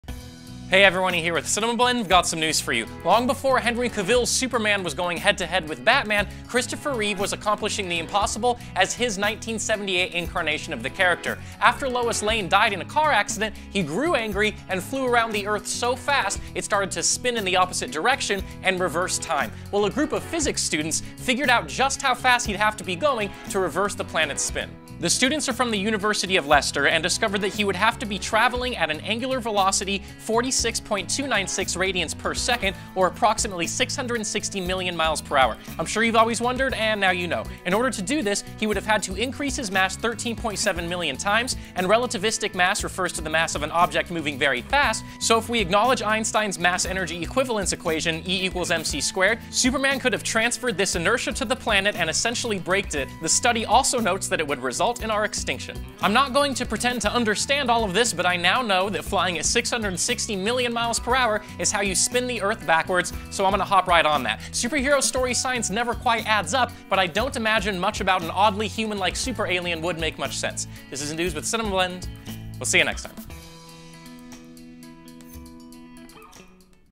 How Fast Superman Would Actually Need To Fly To Reverse The Earth’s Spin
In 1978 Christopher Reeve as Super-Man flew around the world so fast it reversed the Earth's rotation, and now we know just how fast he must have been going.